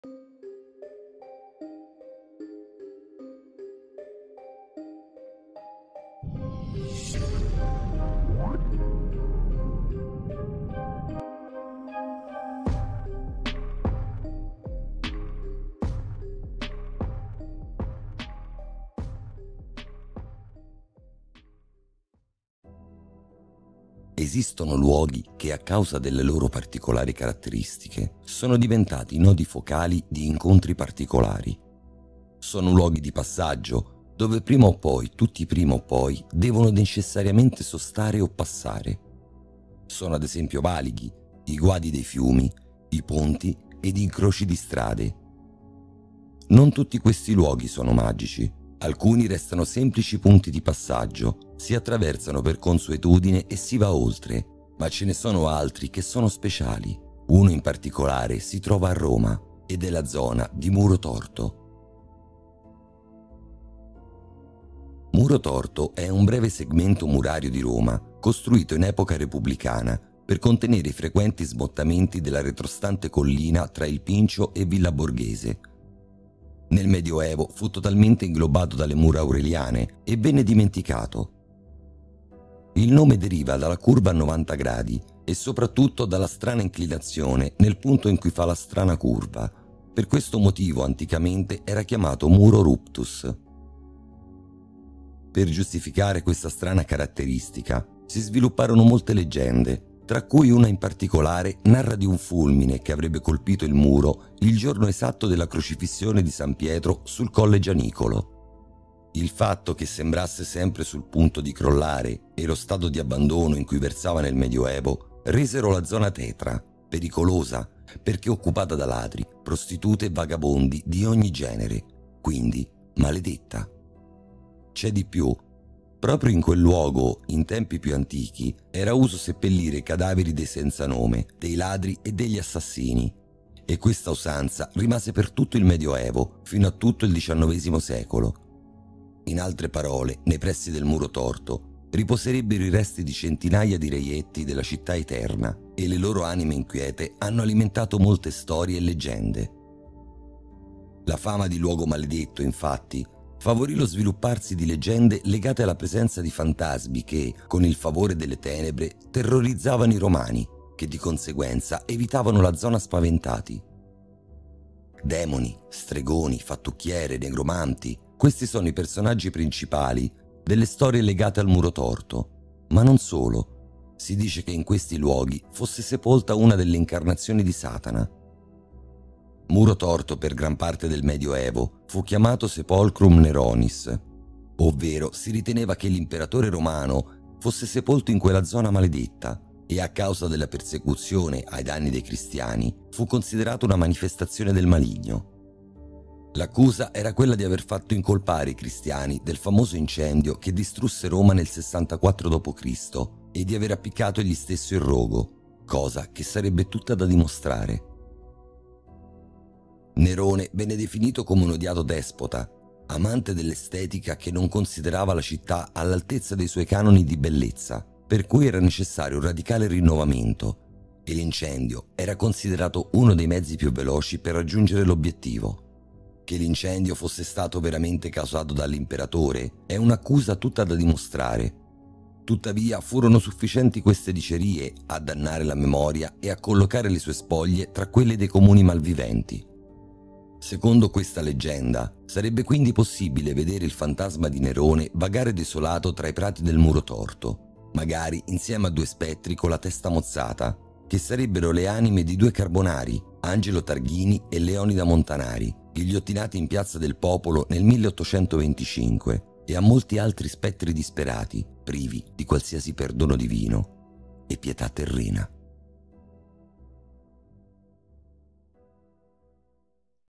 I Fantasmi di muro torto - Storie Inspiegabili
Esistono luoghi che, a causa delle loro particolari caratteristiche, sono diventati nodi focali di incontri particolari. Sono luoghi di passaggio dove prima o poi tutti prima o poi devono necessariamente sostare o passare, sono un esempio i valichi, i guadi dei fiumi, i ponti ed incroci di strade.